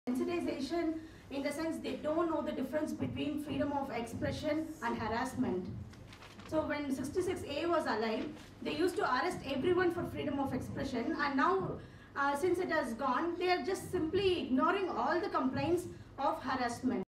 Kiruba Munusamy, Advocate, Supreme Court of India
Kiruba Munusamy, Advocate, Supreme Court of India: After the repeal of Section 66A, police officials are claiming they cannot book cases of online harassment.